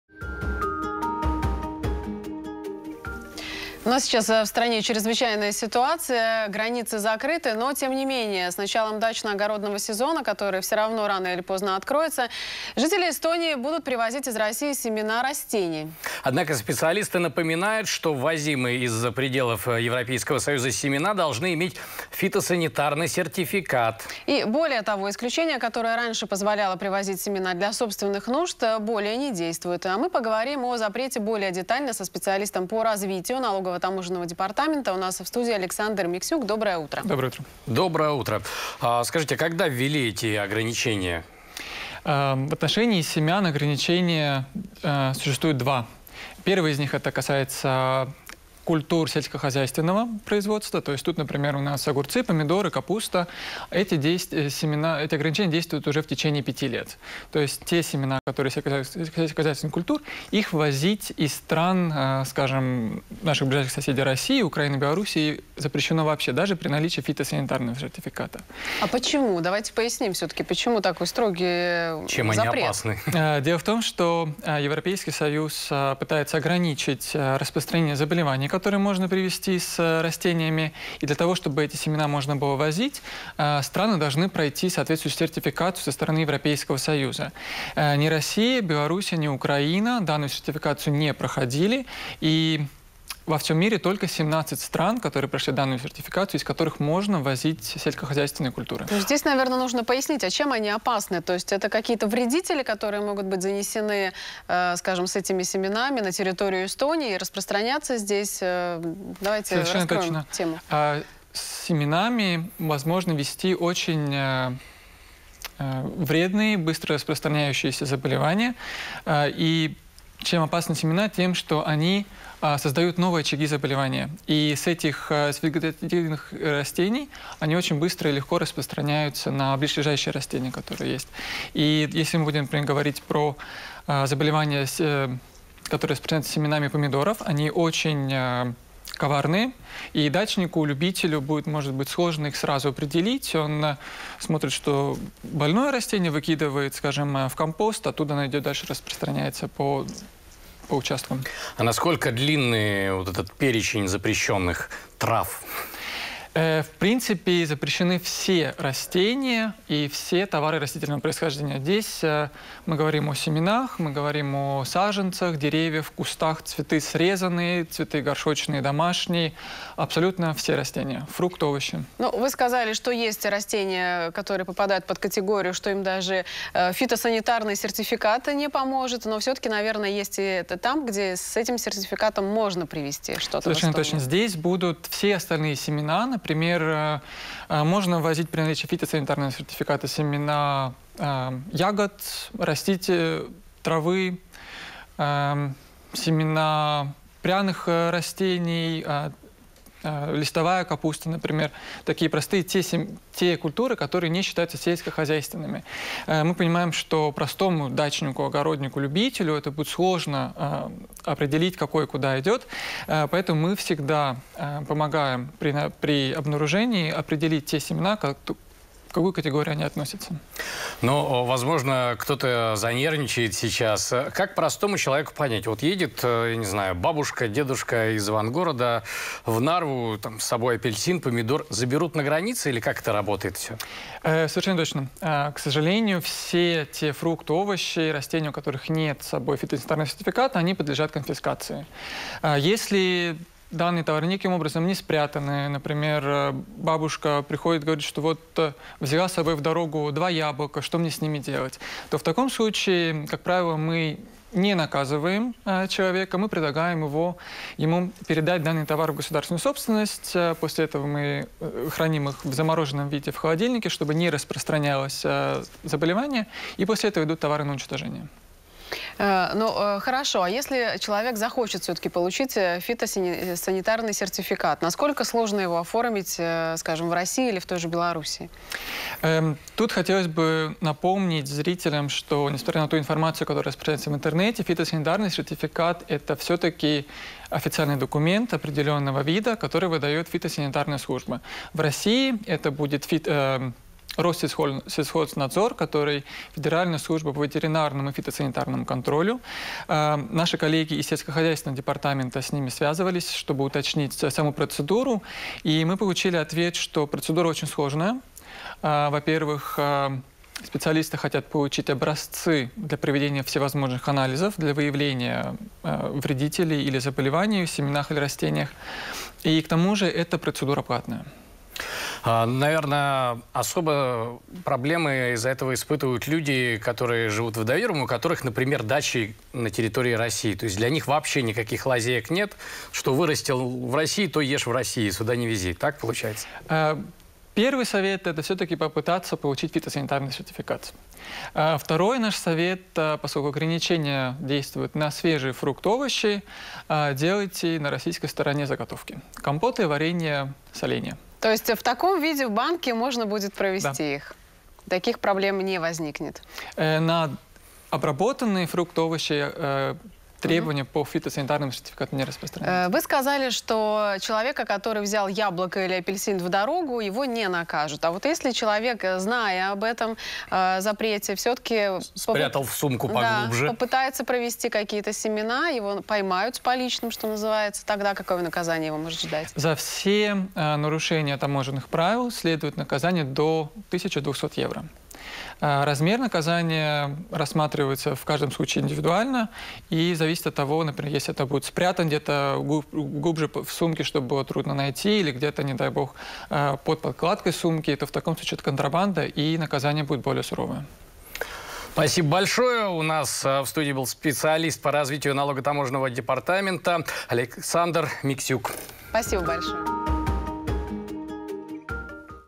Жители Эстонии игнорируют запрет на ввоз семян из России
С началом дачно-огородного сезона жители Эстонии все чаще привозят из России семена растений. Однако специалисты напоминают, что ввозимые из-за пределов Европейского союза семена должны иметь фитосанитарный сертификат. Более того, исключение, позволяющее привозить семена для собственных нужд, более не действует, поговорим о запрете более детально со специалистом по развитию Налогово-таможенного департамента, у нас в студии Александр Миксюк.